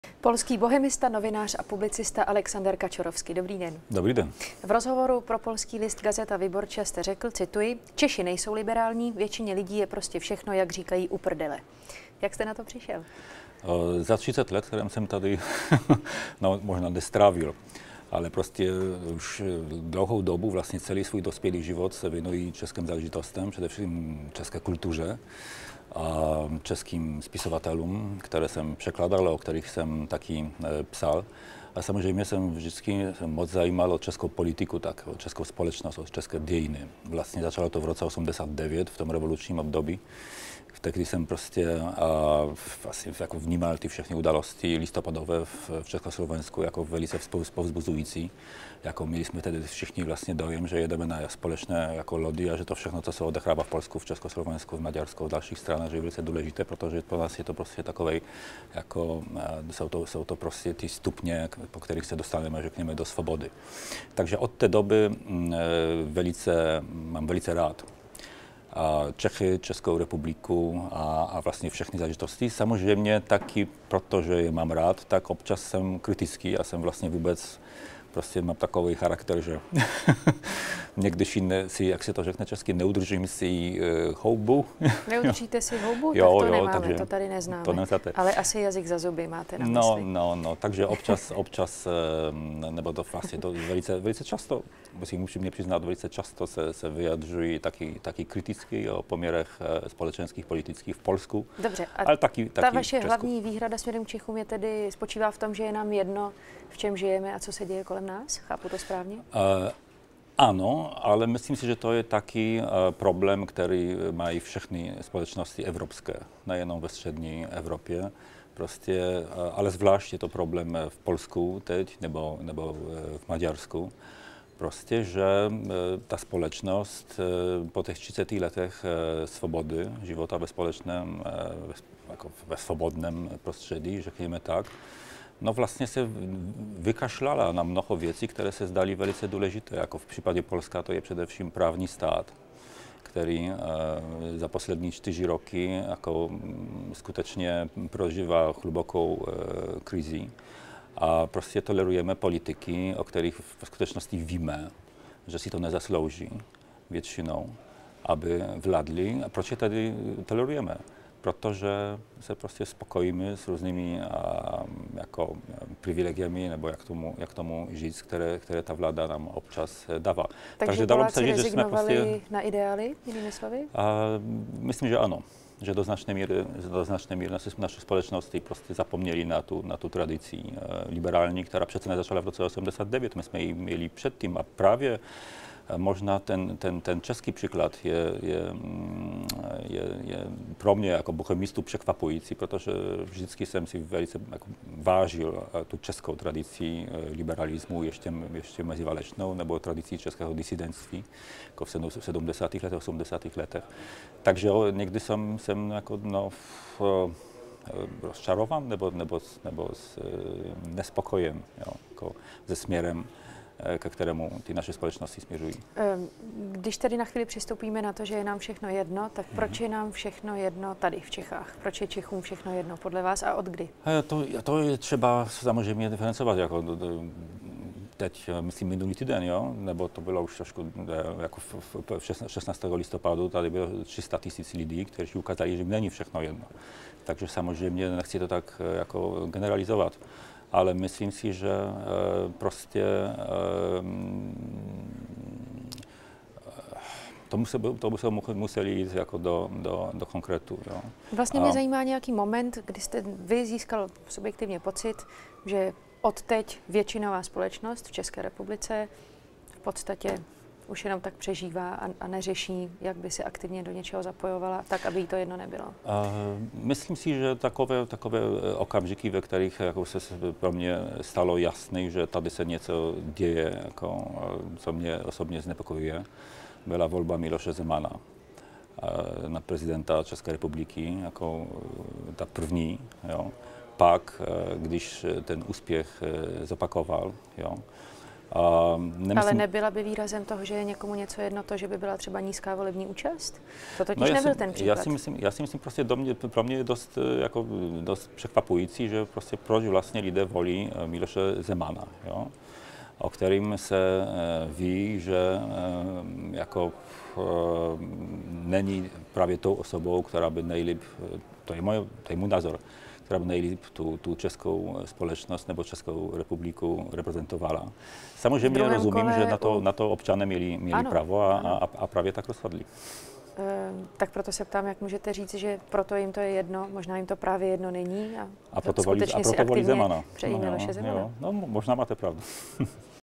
Češi mě občas štvou, už nejsou tak liberální a na dost věcí kašlou, říká Kaczorowski
Překvapuje mě, že Češi volí Miloše Zemana. V Polsku jsme na tom ale ještě hůř, vláda vystupuje proti ústavě a z veřejnoprávní televize udělala stranickou, říká polský bohemista Aleksander Kaczorowski.

Co ho na Češích štve? A proč jsou na tom Poláci v mnoha směrech hůř? Na celý rozhovor se podívejte zde